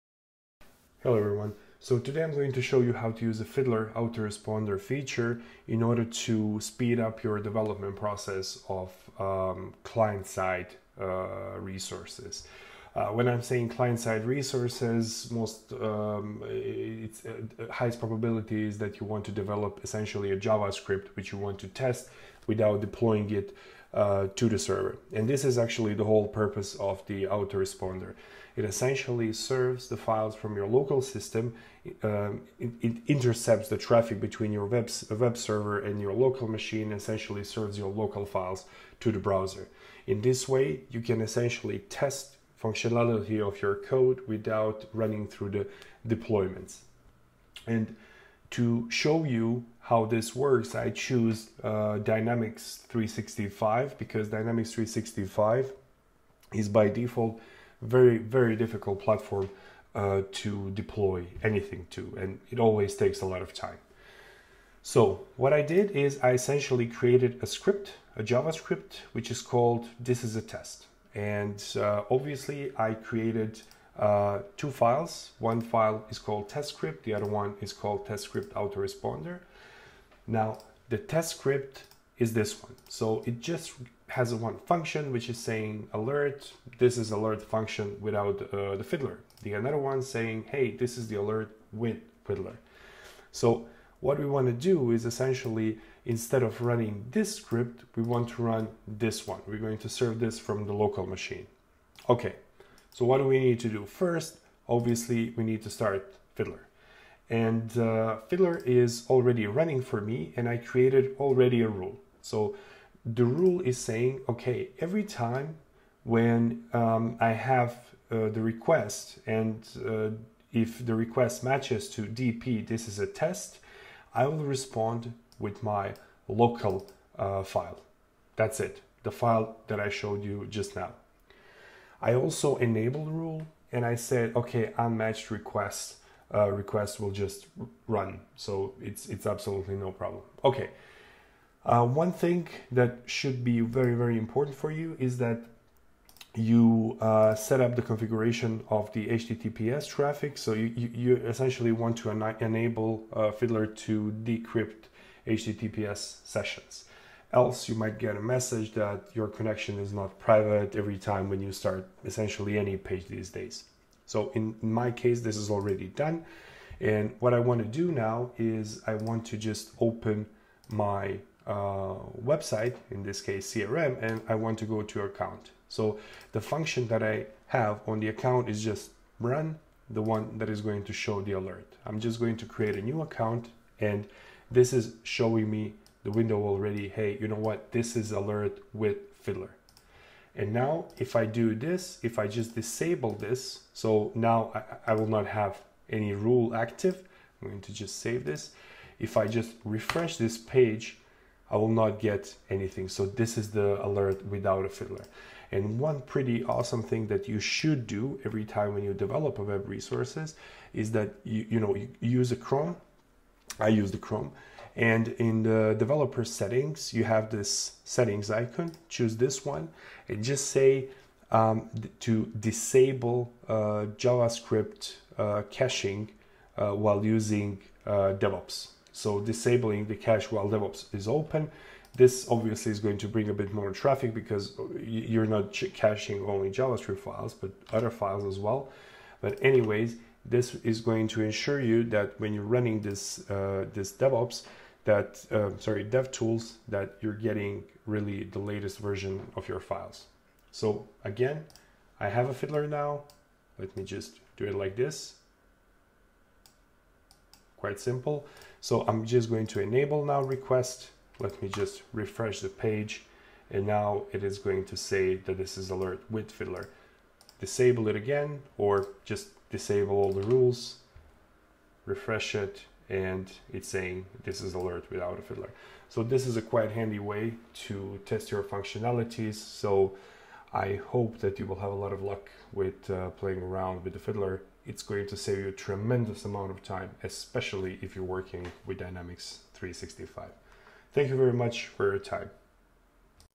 Building & Debugging Web faster with Autoresponder
This video explains how to use Fiddler Autoresponder feature to server local files to the web browser. Example is done in the Dynamics 365 which can be terrible difficult to deploy to.
Using Autoresponder, you don't need to deploy your code to the Web application to test functionality, but you use your locally developed files to test functionality.
Now you can deploy your code when you are happy with it :)